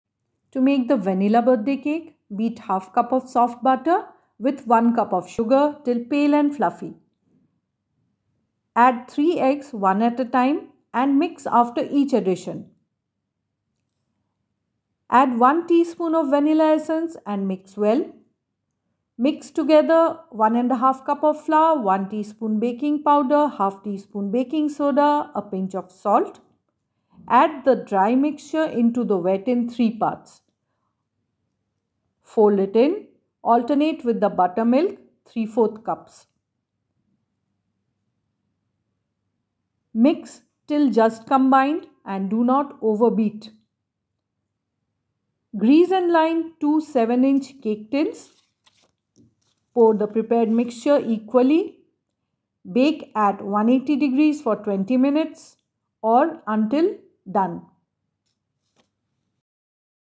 Easy Vanilla Birthday Cake Recipe| Easy Butter Cake Recipe|Never Fail
It's still the baking season right?
Watch an Easy Vanilla Birthday Cake Recipe
How to make vanilla butter cake?
This never fail vanilla butter cake recipe is for you.
The easy butter cake recipe has eggs and buttermilk in it. The surprisingly simple recipe is good for a beginner as well as an expert.
If you are planning a birthday party this recipe will surely deliver. A home baked butter cake anyday…

VANILLA BIRTHDAY CAKE RECIPE  Serves 8 to 10

Ingredients
DRY INGREDIENTS
All Purpose Flour/Maida – 1 ½ cup
Baking pdr – 1 tsp

Superfine/Castor Sugar – ¾ cup
Butter (soft) – ½ cup
Eggs – 3
Vanilla Essence – 1 tsp
Butter milk – ¾ cup

Method:
1. Mix the DRY INGREDIENTS very well, using a whisk. Keep aside.
2. In a mixing bowl, beat the butter and sugar till pale and fluffy. Add in the eggs one at time. Beat to incorporate after each addition.
3. Add the essence and beat it.
4. Add the dry ingredients (in three parts) to the egg mixture, alternating with the butter milk. Mix by folding method gently after each addition.
5. Grease and line two seven inch round baking tins. Pour the prepared batter into the two tins, divided equally.
6. Tap the tins to remove any air bubbles.
7. Bake in a preheated oven at 180 C for 20 minutes or until a skewer inserted comes out clean.
8. Remove from oven and cool completely on a wire rack.

*NOTE: Oven temperatures differ, so check after 10 mins of baking. If required, turn the temperature down a little.

(Rinki Mukerjee)

Some recipes that will interest you...

Recipes that will interest you...

Let me know how it turned out in your kitchen… I would love to hear from you.

Till the next post …STAY SAFE…

Rinki Mukerjee